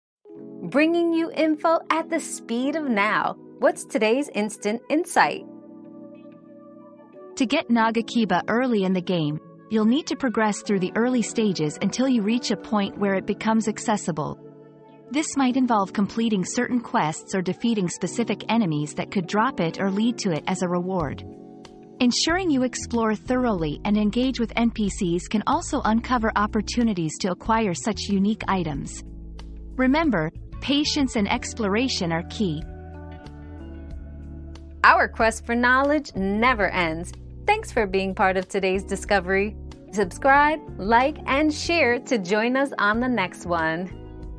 Where can I get Nagakiba early?
Unlocking Nagakiba: Early Game Tips 👉 Nagakiba Tips 👉 Learn how to get Nagakiba early in the game by progressing through stages, completing quests, defeating enemies, and engaging with NPCs. Discover the key to unlocking this unique item with patience and exploration.

Laura S. Harris (2024, March 28.) Where can I get Nagakiba early?
    🌐 AskAbout.video